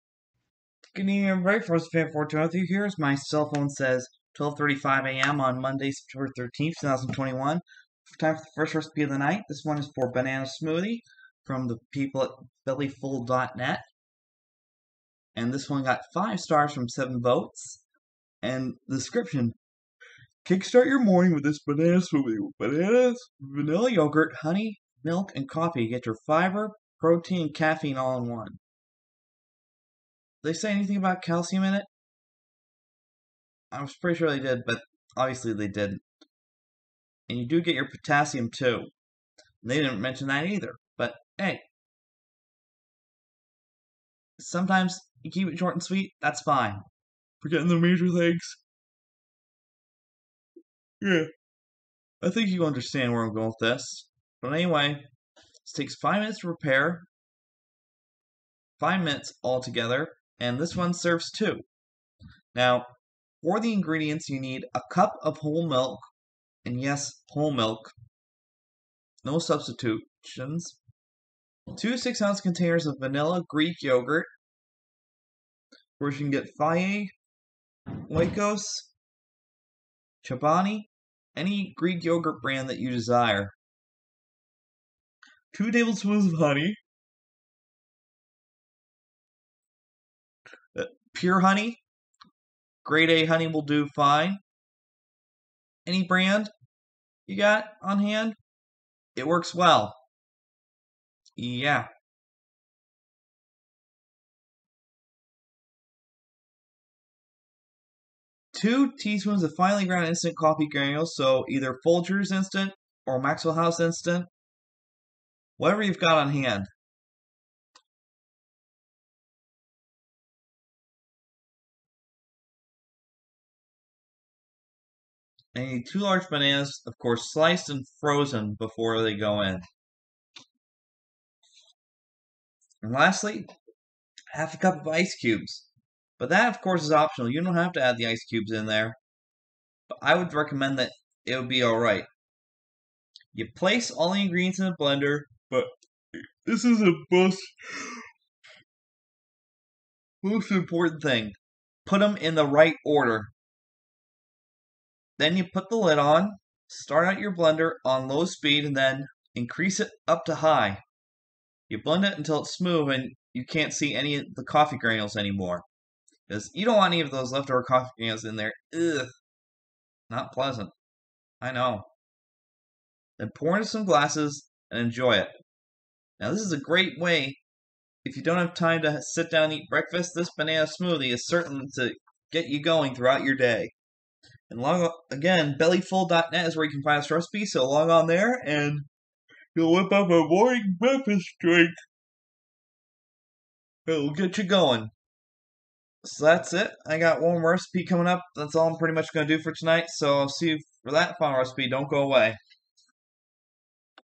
How To Make A Banana Smoothie (Quick Morning Drink)
Want to make a great breakfast drink that doesn't require a lot of work or ingredients? well, this one is for you. it's a Banana Smoothie from BellyFull.net and it's a good one! it's got Greek Vanilla Yogurt, Honey, Bananas, Whole Milk, Instant Coffee Granules and blended until it's smooth! it's a perfect way to get Potassium, Fiber, Protein and Caffeine to start your day. I do hope you try this one!